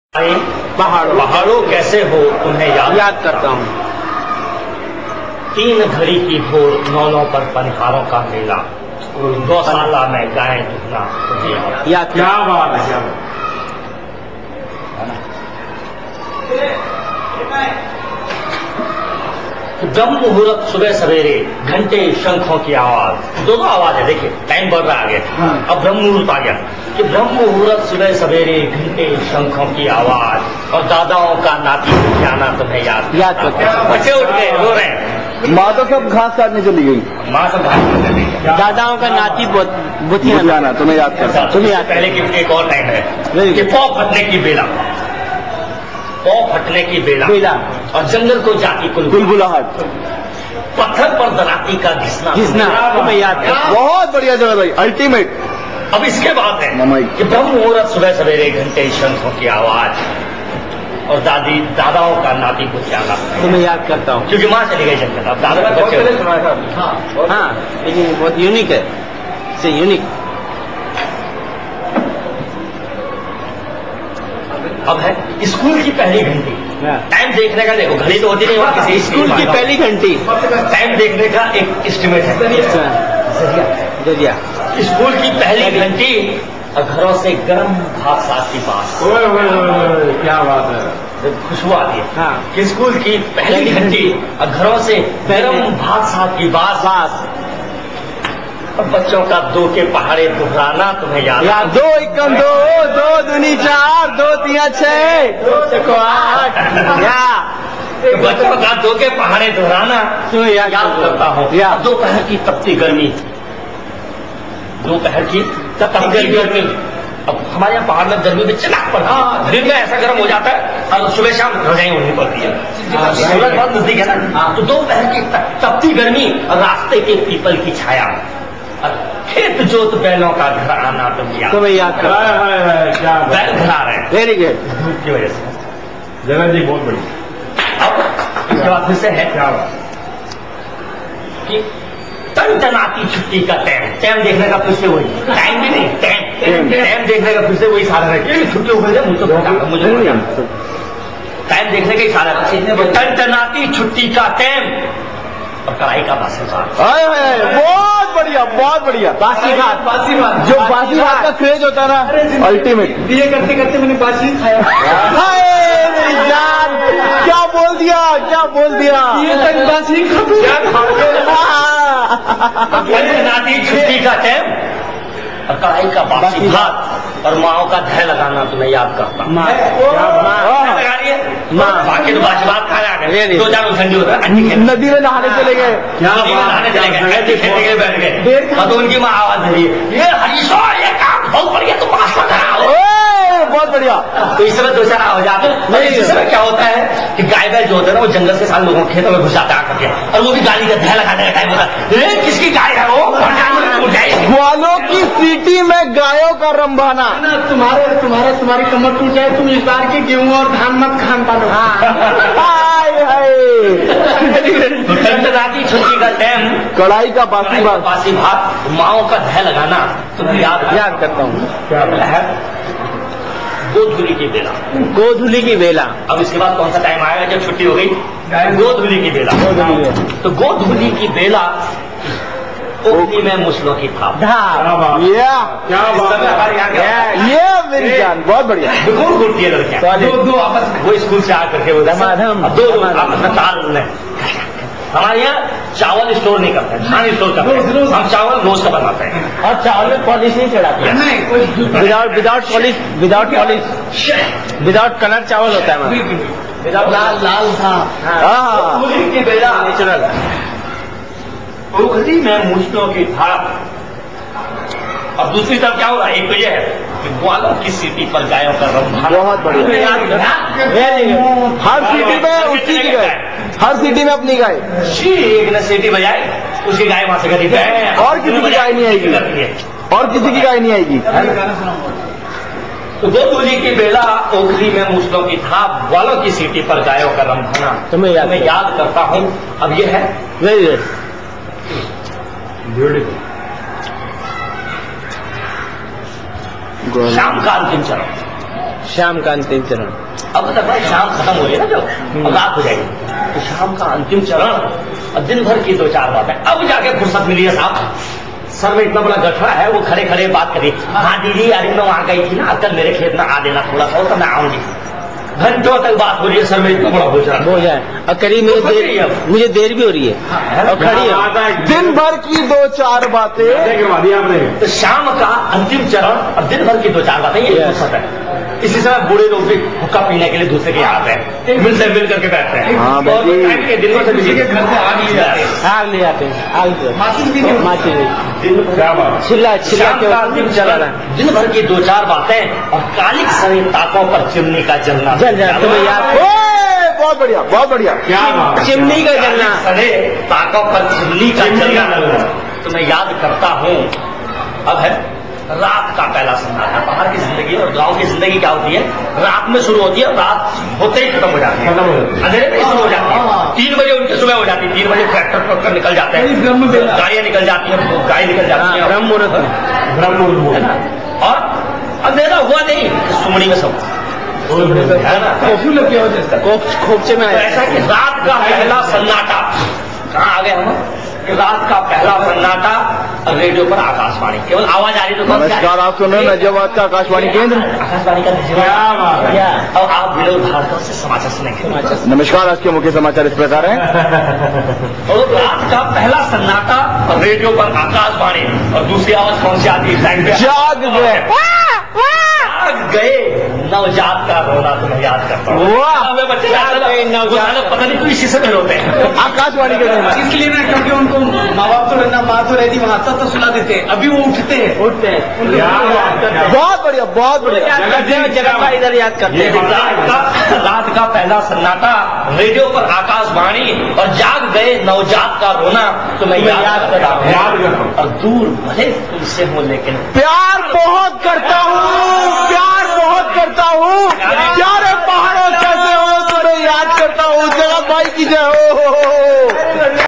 Pyare Pahado Kaise Ho
A poem by Jagat Kishor on life in Pahad (mountains) Uttaranchal of India.

प्यारे पहाड़ो कैसे हो, तुम्हें याद करता हूँ।

पौ फटने की बेला और जंगल को जाती कुलबुलाहट,
पत्थर पर दराँती का घिसना, तुम्हें याद करता हूँ।

ब्रह्म मुहूरत सुबह सवेरे, घण्टे-शंखों की आवाज,
दादाओं का नाती बुतियाना, तुम्हें याद करता हूँ।

स्कूल की पहली घण्टी और घरों से गरम भात-साग की बास,
बच्चों के दो के पहाड़े दुहराना, तुम्हें याद करता हूँ।

दोपहर की तपती गर्मी और रास्ते के पीपल की छाया,
खेत-जोत बैलों का घर आना, तुम्हें याद करता हूँ।

टनटनाती छुट्टी का टैम और कढाई का बासी भात,
माँओं का धै लगाना, तुम्हें याद करता हूँ।

गोधूलि की बेला, ओखली में मूसलों की थाप,
ग्वालों की सीटी पर गायों का रम्भाना, तुम्हें याद करता हूँ।

शाम का अंतिम चरण, दिन भर की दो-चार बातें,
कालिख सनी ताकों पर चिमनी का जलना, तुम्हें याद करता हूँ।

रात का पहला सन्नाटा, रेडिओ पर आकाशवाणी,
जाग गये नवजात का रोना, तुम्हें याद करता हूँ।

दूर भले तुमसे हूँ लेकिन, प्यार बहुत करता हूँ,